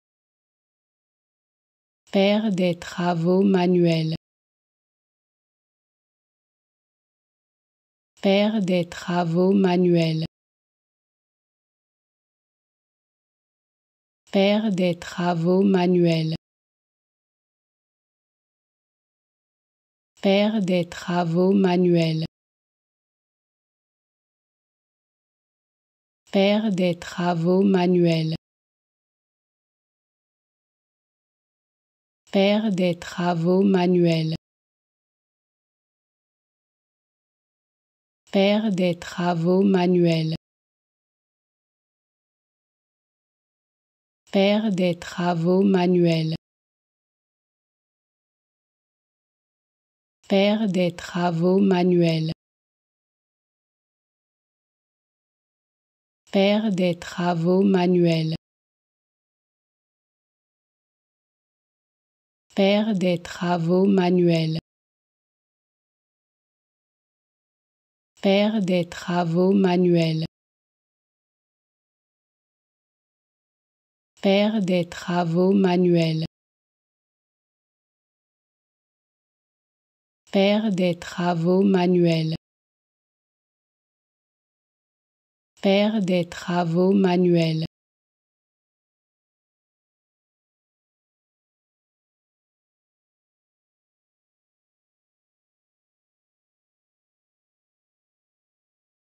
How to Pronounce ''Faire des travaux manuels'' (Doing manual work) Correctly in French
Learn how to say and properly pronounce ''Faire des travaux manuels'' in French with this free pronunciation tutorial.